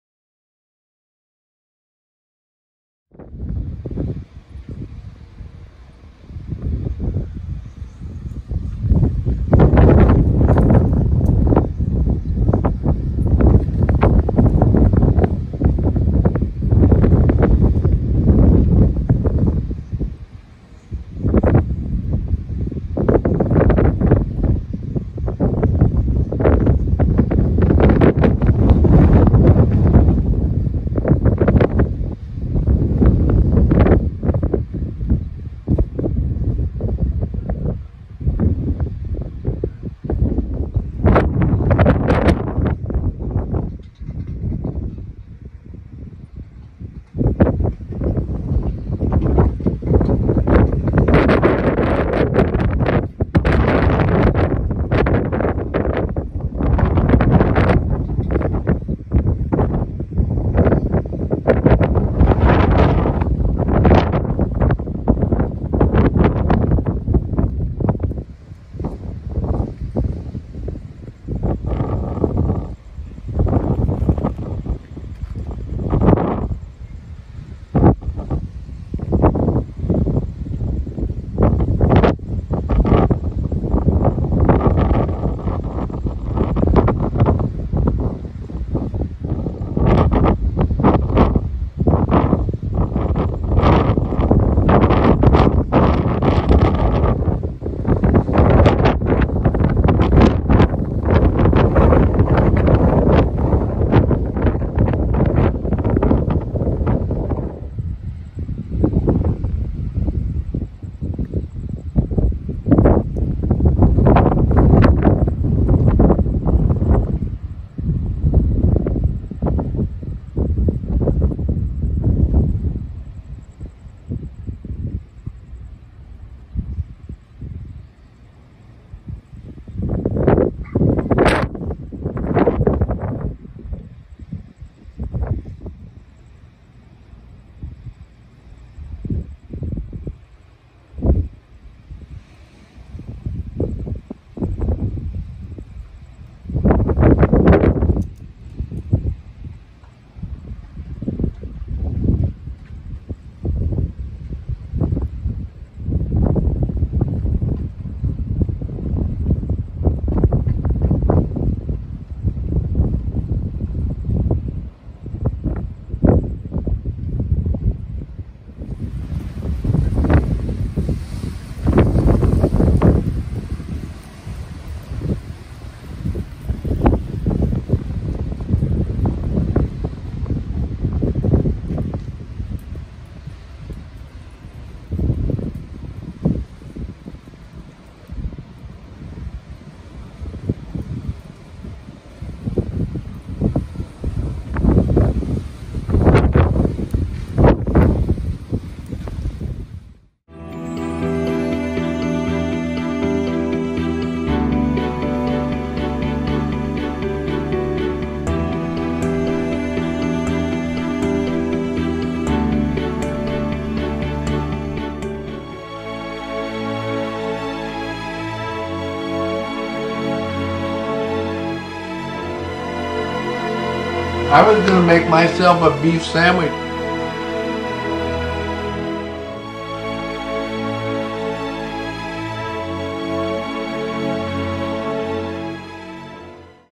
ElECtRiCAl stoRm
Wild and wooly electrical storm in Lake Havasu City, in Arizona, in USA, in Earth... or on Earth?  Snicker Galaxy...no it's the Milky Way Galaxy, cuz Almond Joy has nots; Mounds nut so much.  You get the jizst, right?